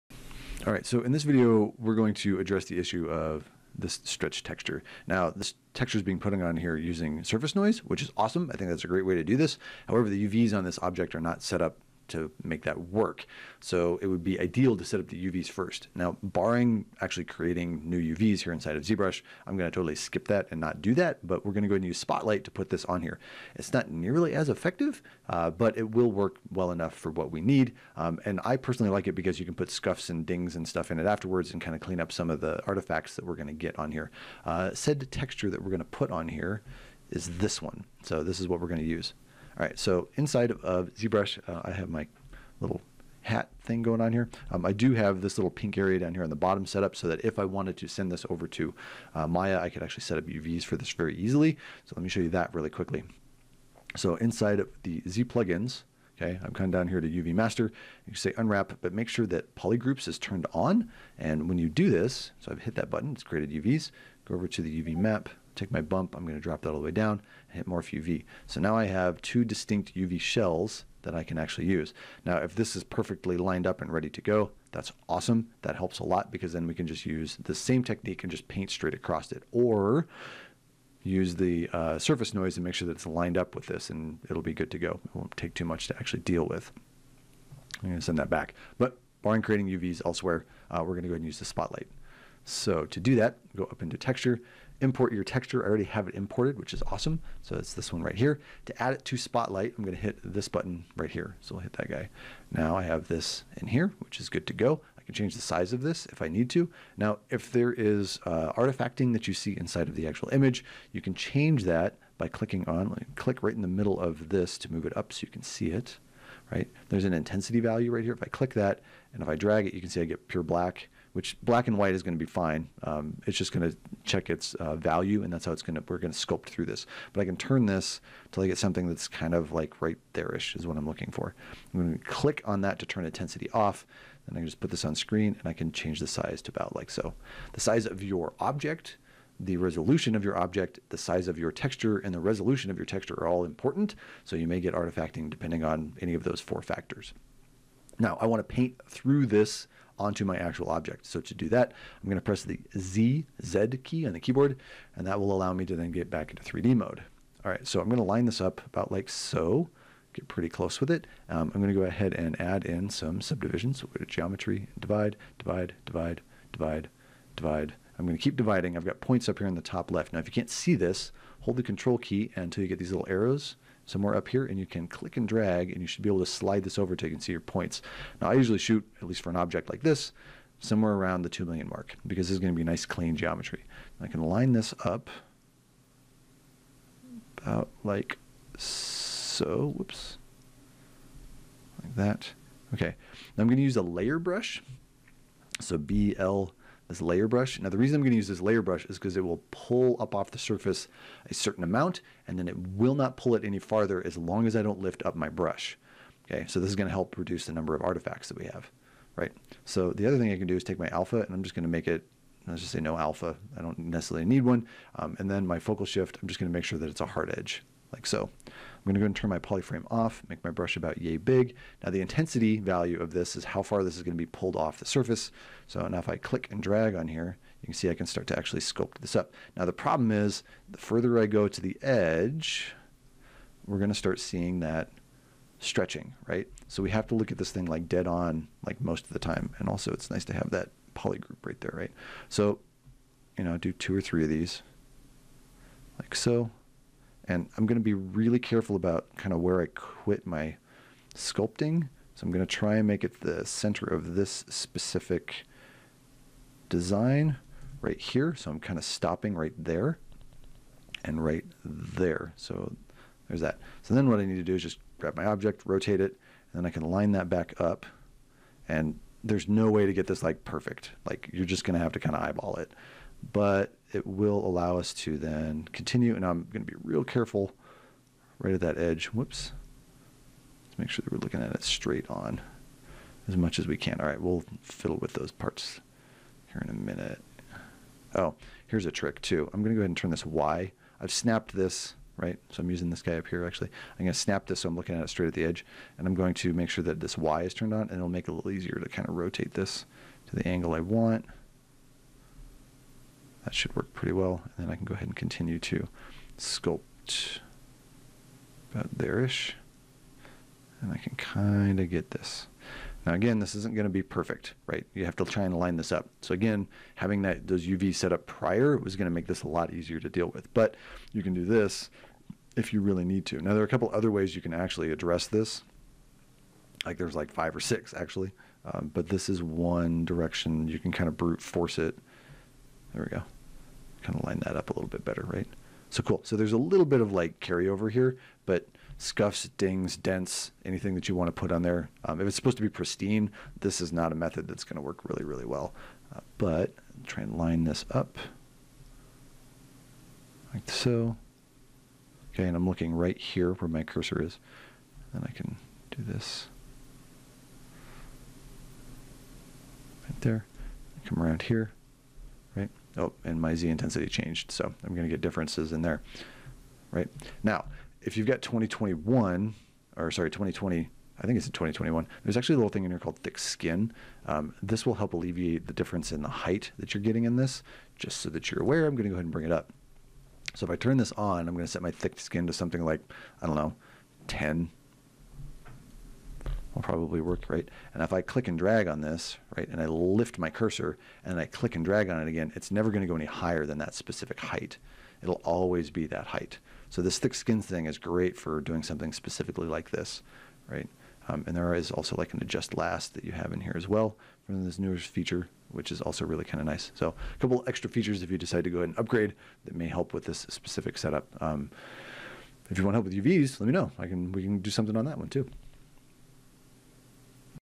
Using Spotlight to apply a pattern in ZBrush
This is one way to apply a pattern in ZBrush. It's rather brute force, but it will work.